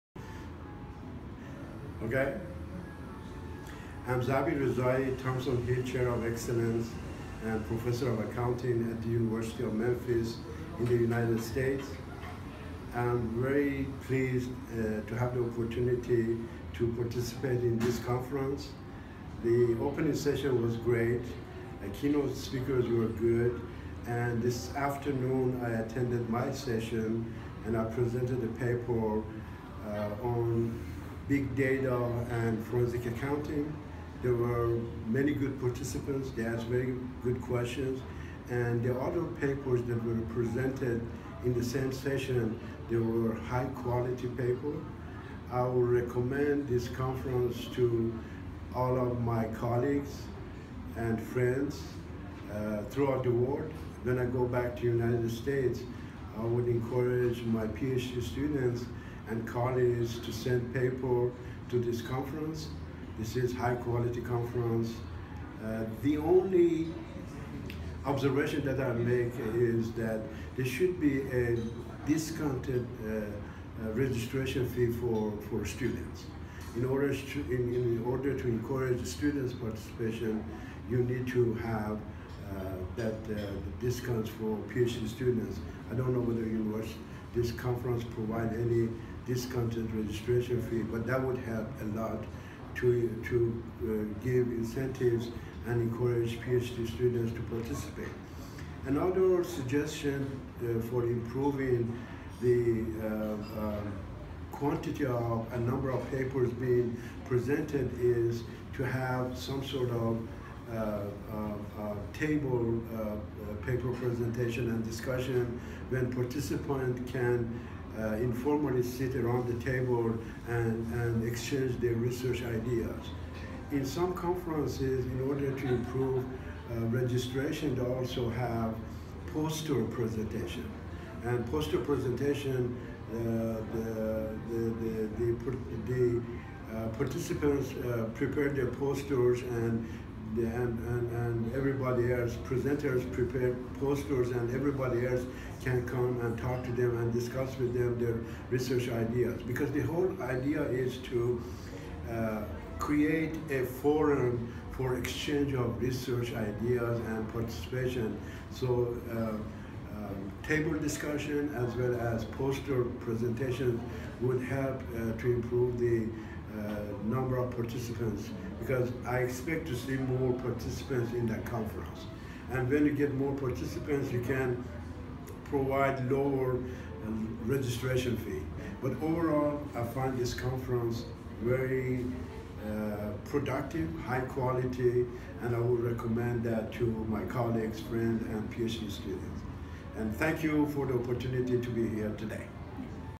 Zabihollah Rezaee at AF Conference 2017 by GSTF
Zabihollah Rezaee

Testimonial: Zabihollah Rezaee at the Accounting and Finance - AF

Conference 2017 by Global Science and Technology Forum – GSTF Singapore